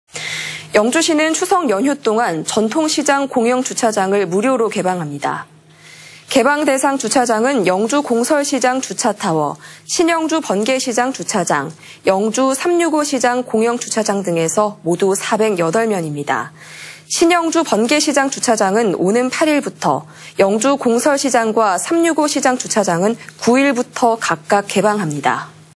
영주시, 추석 연휴 전통시장 주차장 무료 개방 / 안동MBC
2022/09/06 09:45:22    작성자 : 김서현

영주시는 추석 연휴 동안
전통시장 공영주차장을 무료로 개방합니다.
◀ＥＮＤ▶

개방 대상 주차장은 영주공설시장 주차타워,
신영주 번개시장 주차장, 영주365시장
공영주차장 등에서 모두 408면입니다.

신영주 번개시장 주차장은 오는 8일부터,
영주공설시장과 365시장 주차장은 9일부터
각각 개방합니다.